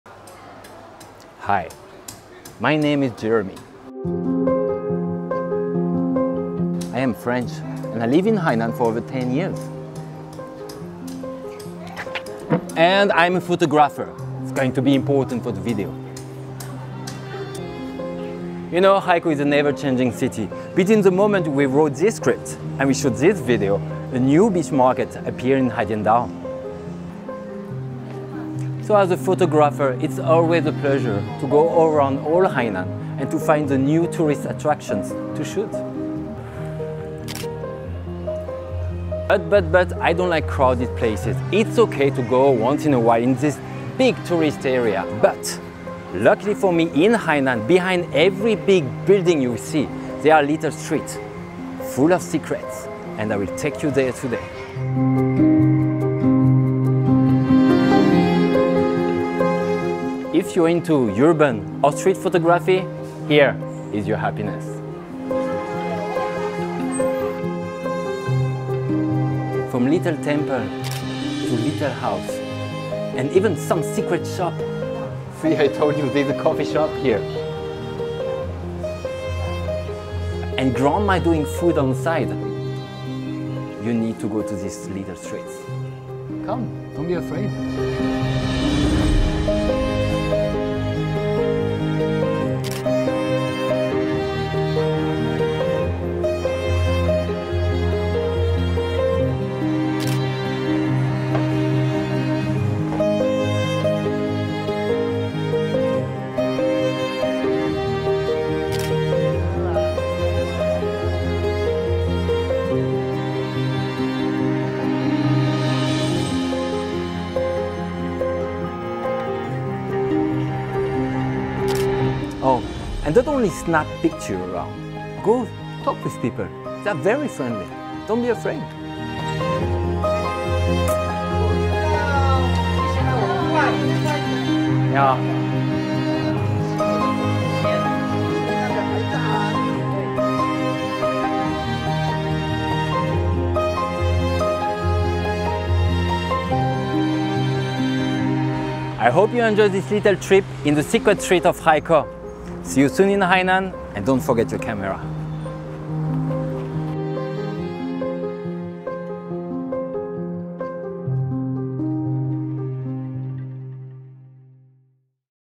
Jeremy Photography
Jeremy, a French photographer living in Hainan for more than ten years, shares his experiences exploring the city through the lens of his camera. He shares his best secrets and favorite places with us.